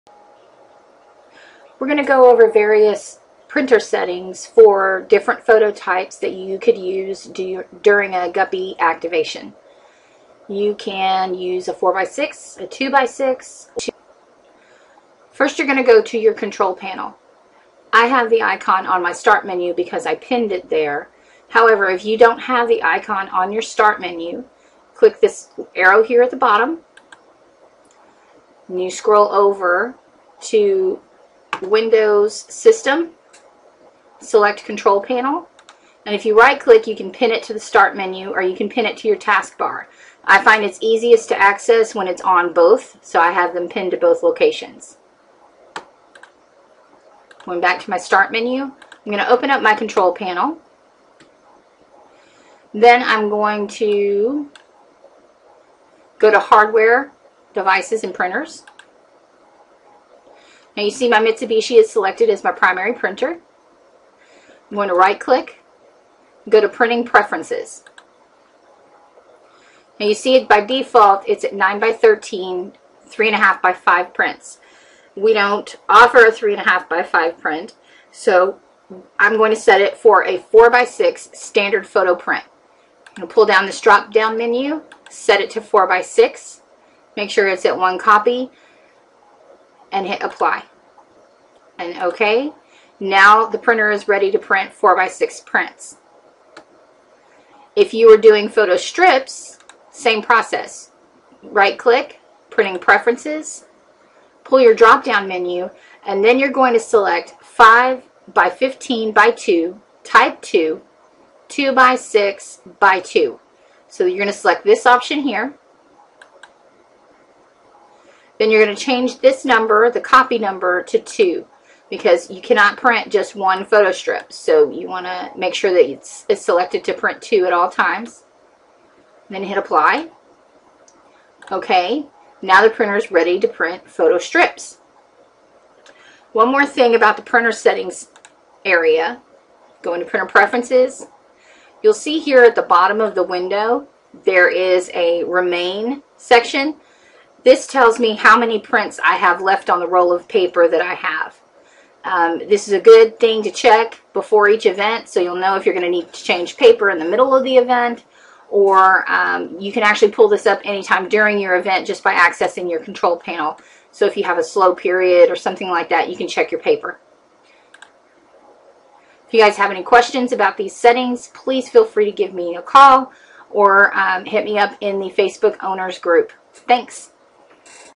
Printer Settings for Mitsubishi CP-D70DW for Guppy Software
Instructions on setting up a Mitsubishi CP-D70DW for use with Guppy Photo Software to produce 4x6 or 2x6 prints.